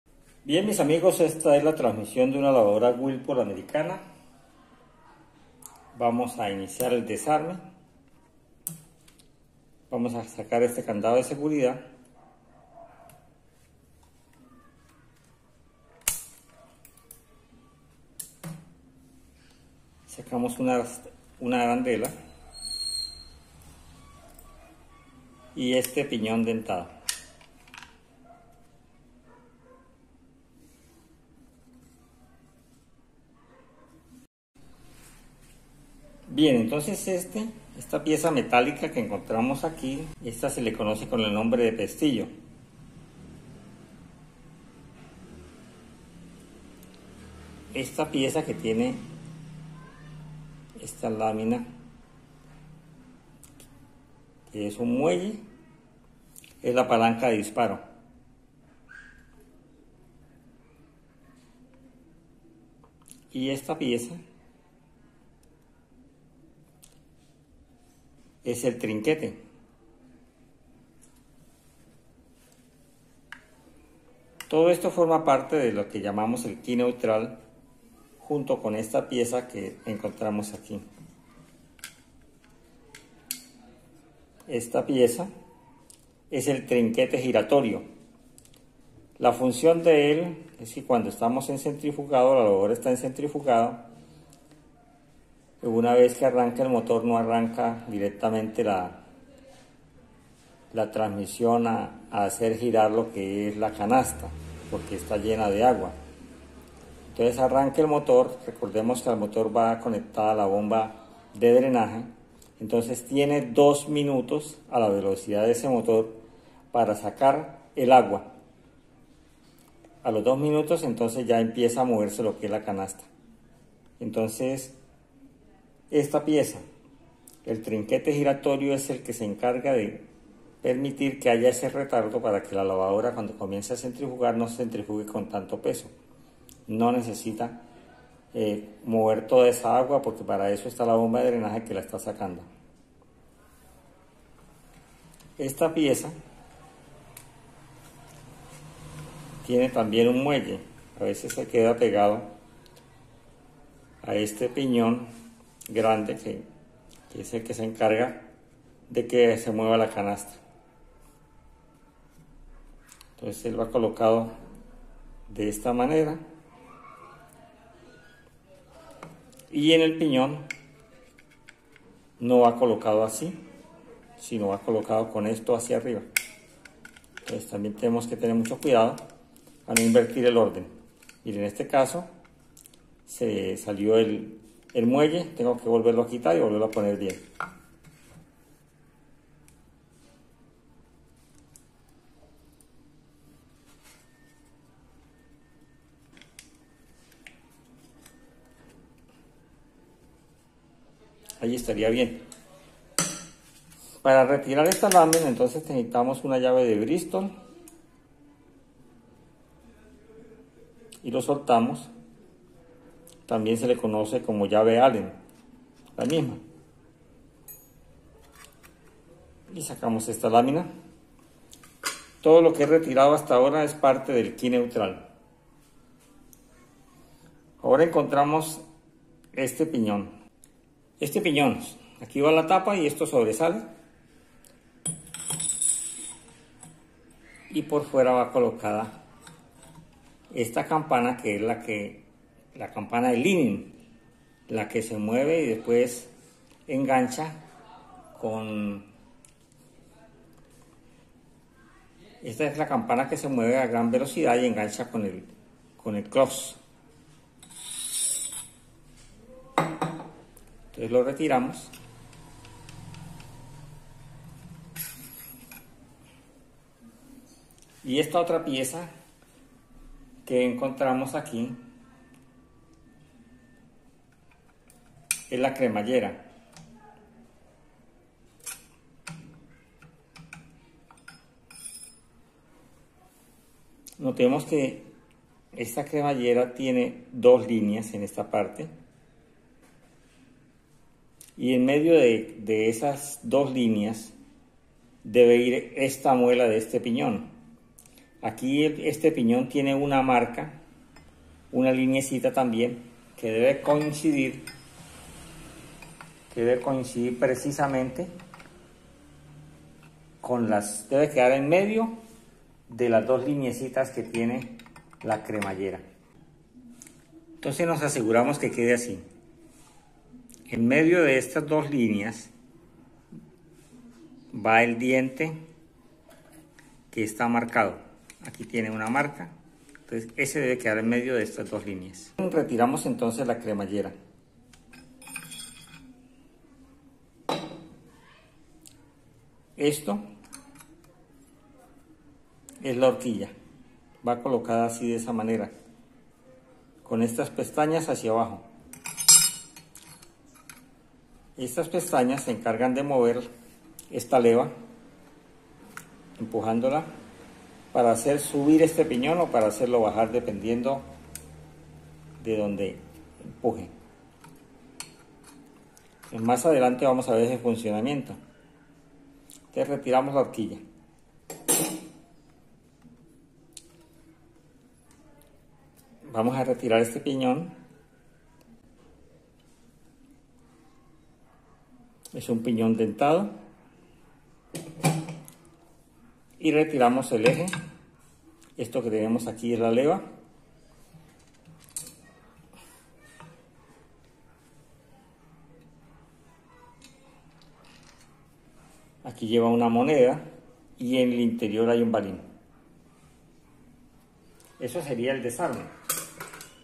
TRANSMISIÓN DE LAVADORA WHIRLPOOL desarme de transmisión de lavadora Whirlpool y Kenmore,explicacion
TRANSMISIÓN DE LAVADORA WHIRLPOOL desarme de transmisión de lavadora Whirlpool y Kenmore,explicacion apaso a paso